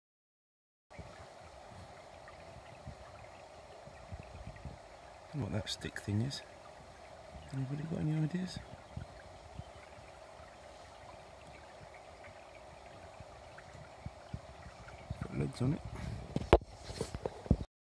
Weird alien bug thing. Found in Brook.
I was looking in the local brook when i spotted this weird alien bug thing.